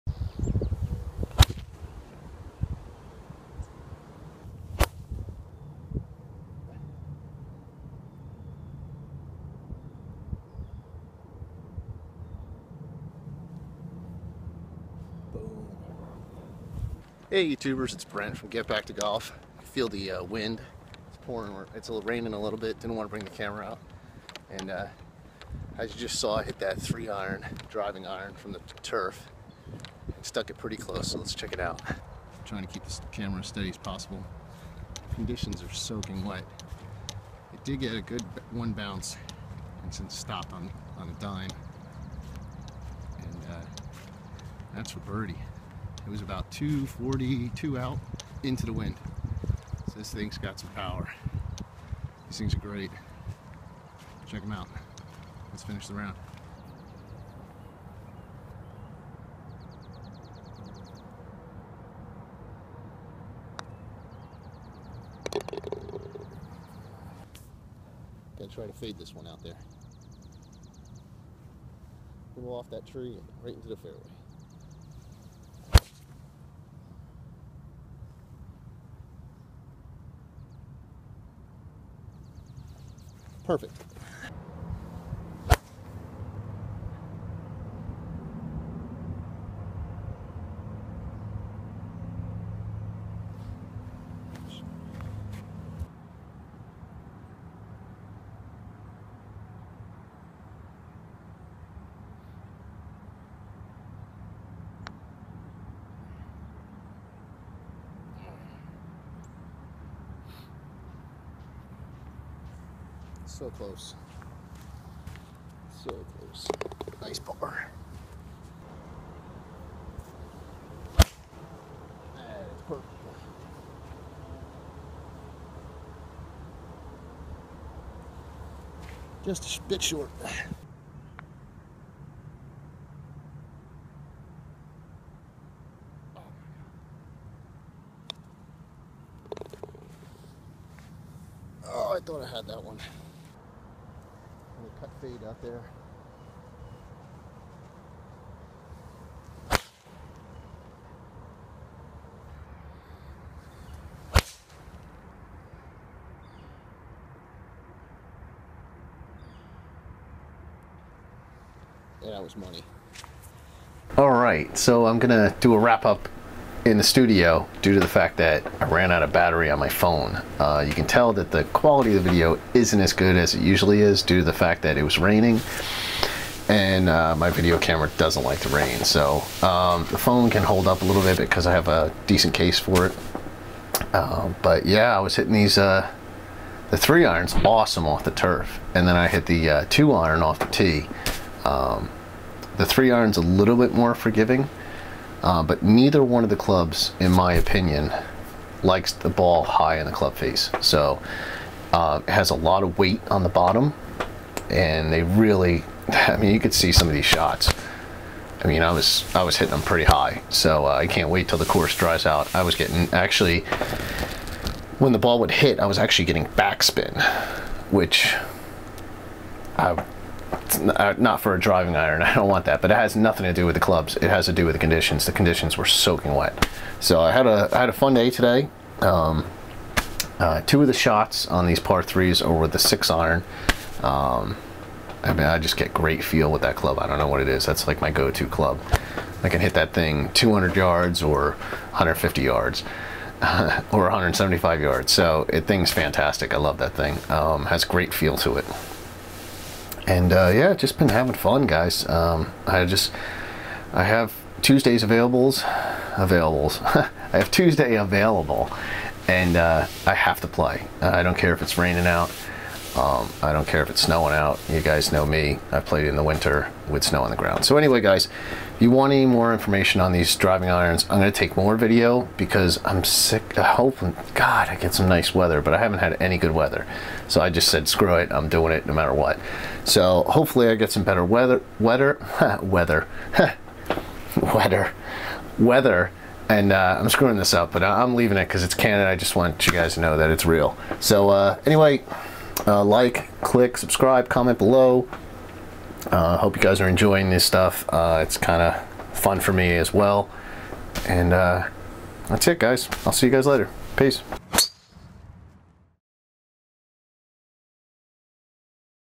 Bombtech Grenade Driving Iron set On course review first shots Golf Vlog 32 2 iron 3 iron
I'm absolutely crushing these Bombtech Grenade driving irons...  These 2 and 3 irons are a perfect fit to my game right now.    They are lots of fun to hit and give me the confidence I need to hit the ball into the fairway.